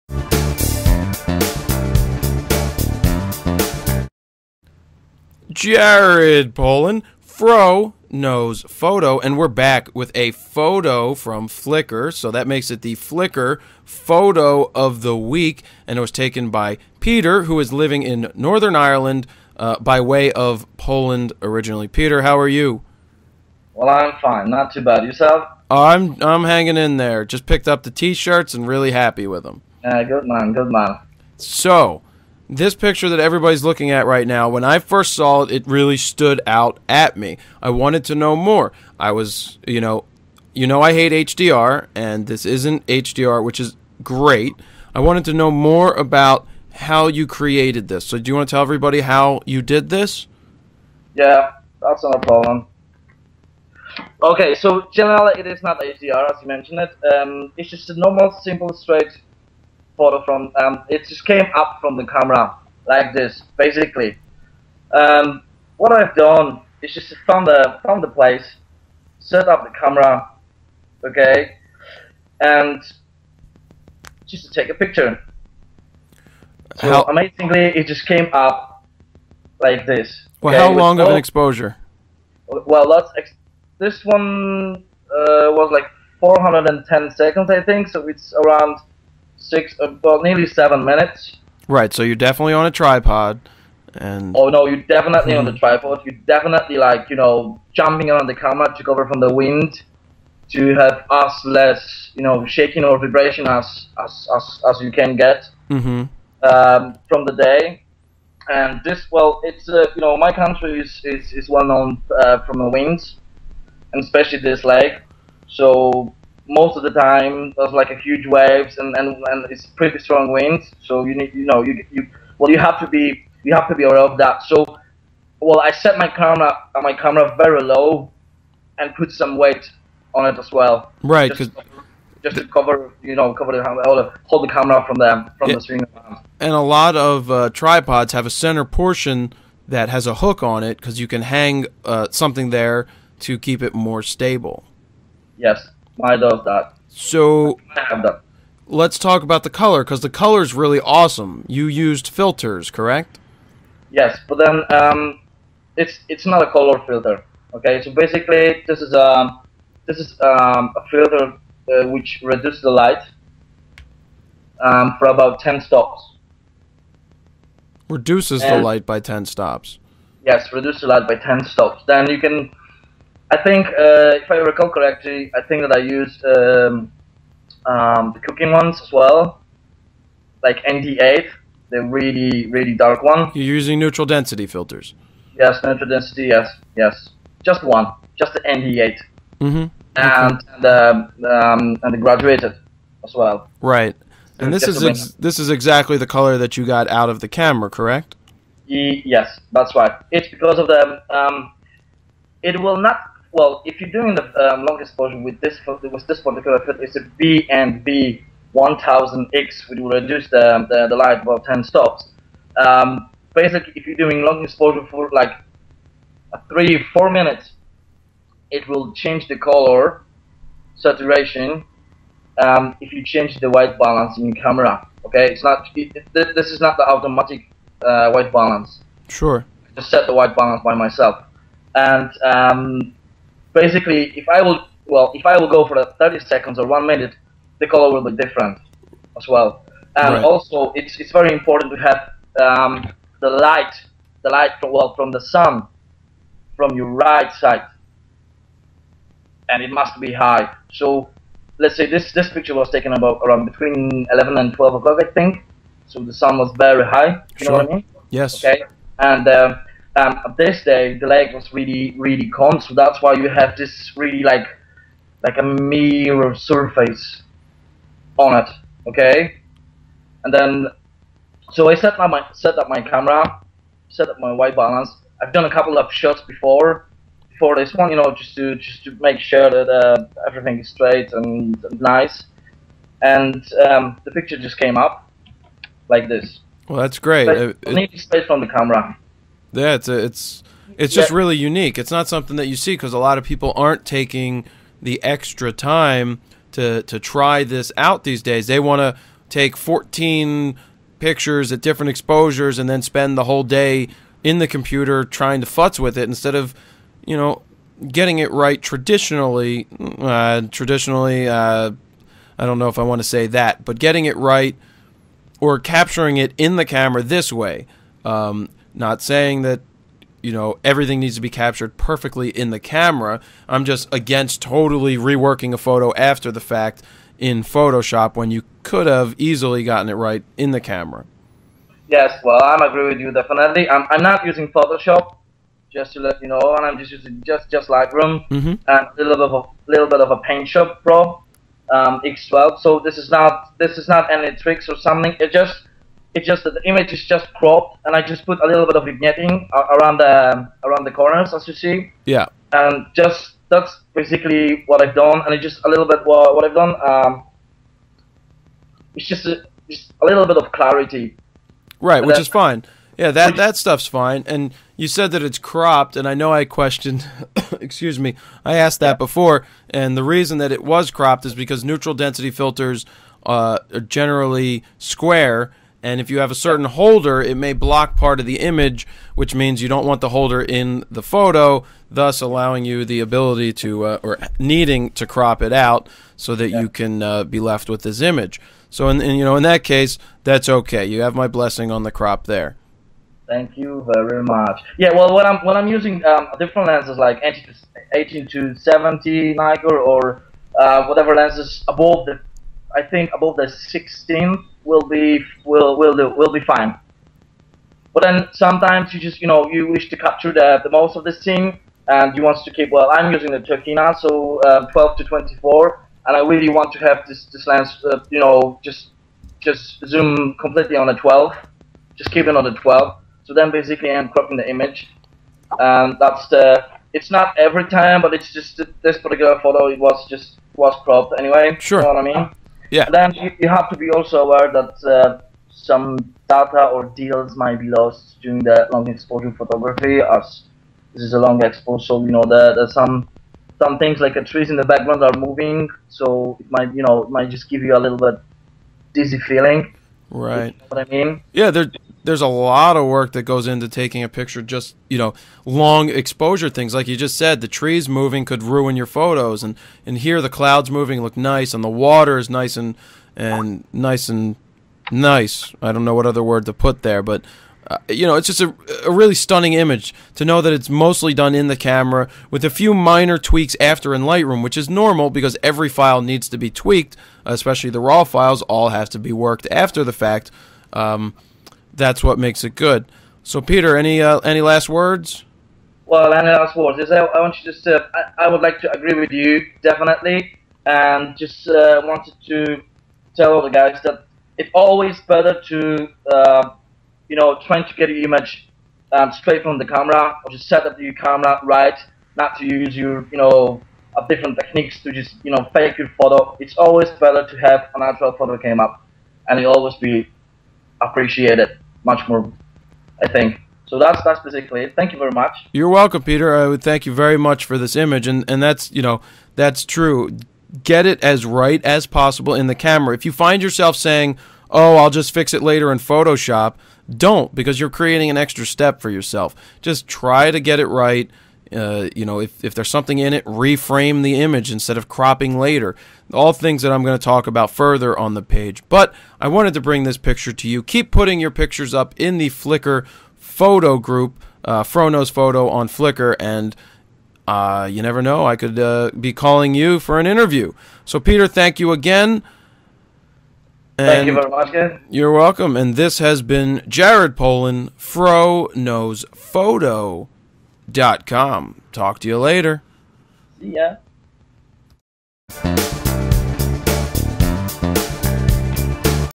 POTW Long Exposure
This weeks flickr photo was taken by Peter Dudek and it is an amazing example of a long exposure photograph.

Take a listen to the interview with Peter where he talks about how he captured this image and how he did very little after the fact to make it better.

It is slightly cropped as his filter holder bled into the image.

Don't forget I select images for the Flickr photo of the week from the FroKnowsPhoto flickr group as well as comment on your work.